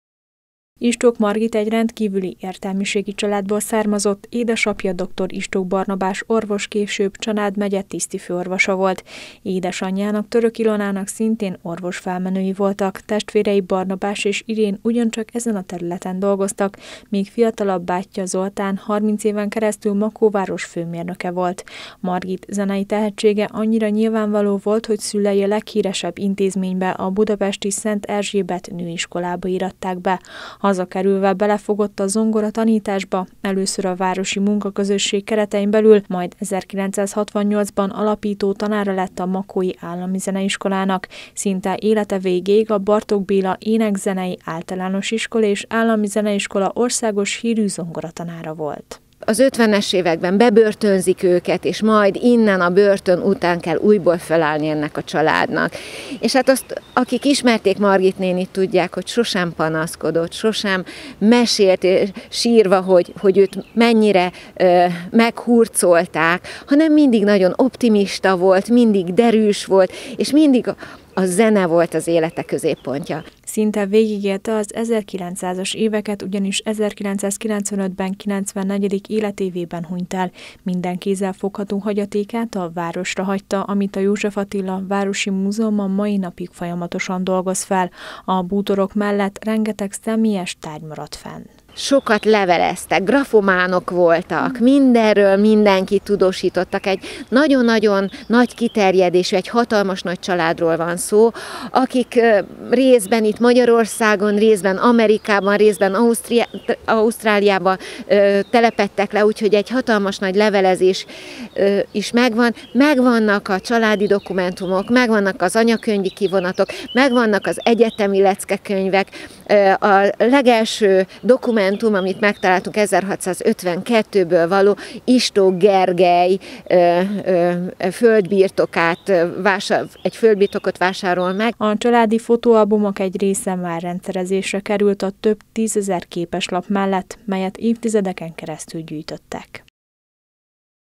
Istók Margit a makói zeneoktatás kiemelkedő alakja volt
A tekintélyes Istók család egykori felmenői között százötven orvost és művészt jegyeztek. Istók Margit zenei tehetsége hamar megmutatkozott, nagyszerűen tanított és korrigált. Végrendeletében házát és minden ingóságát Makó városára hagyta, melyet a József Attila Városi Múzeum tart nagy becsben.